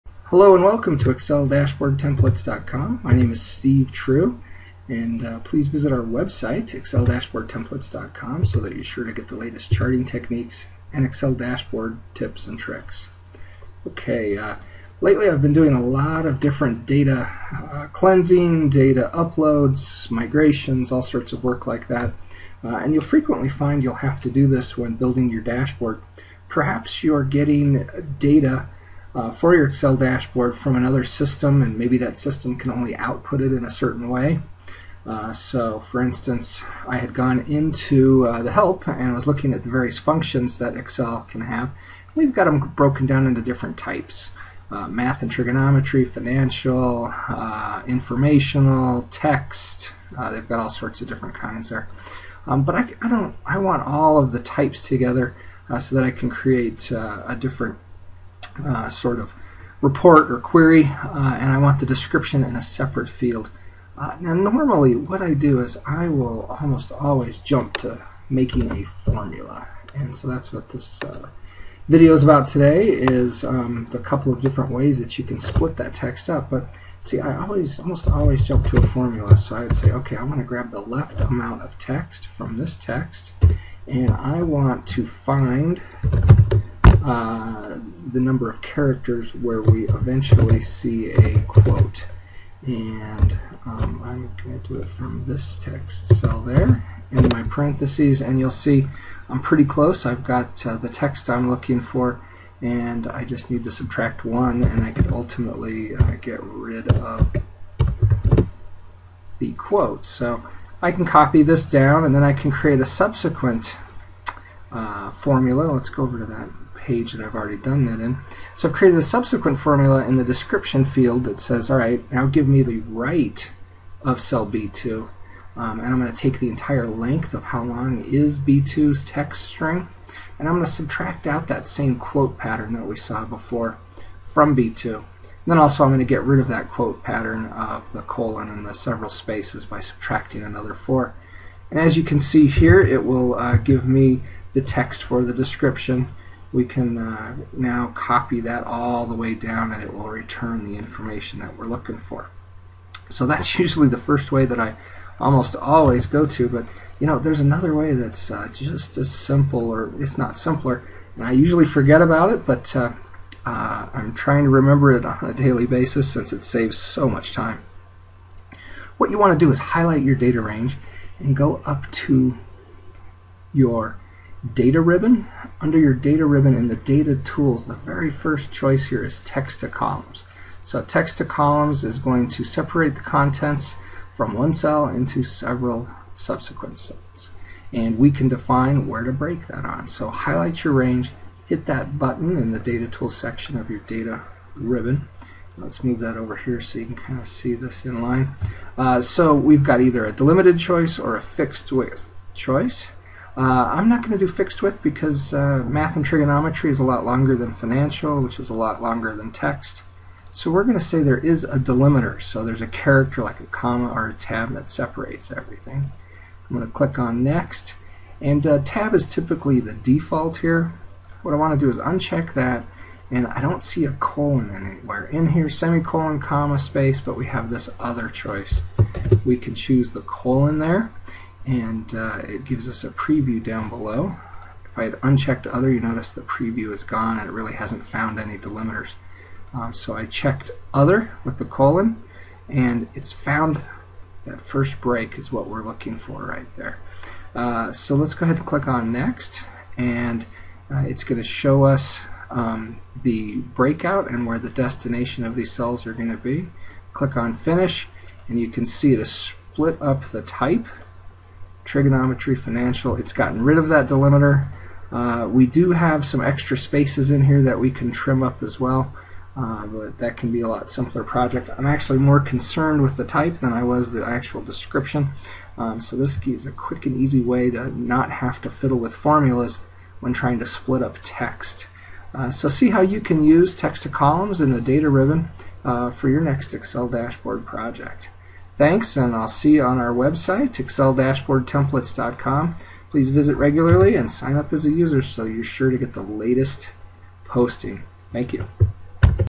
Splitting Cell Text and Numbers Using Formulas And Text to Columns
In most data sets you may need to break up the text into many columns of data.   In my example below, I will show you how to do this on text that has various lengths, so the formulas can be tricky but there is an easy way to do this using the Excel Text to Columns feature.